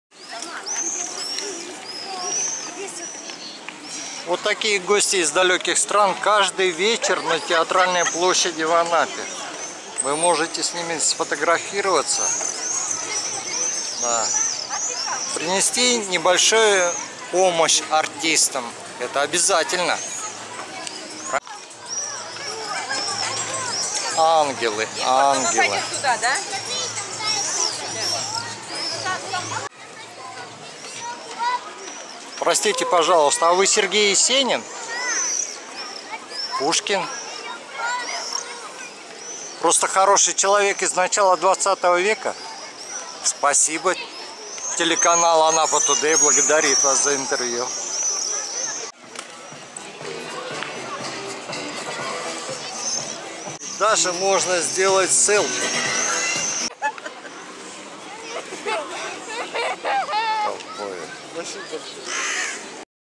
Каждый вечер в Анапе
Примерно с 19 до 23 часов в Анапе на Театральной площади или на Набережной можно сфотографироваться со сказочными живыми героями из фантазий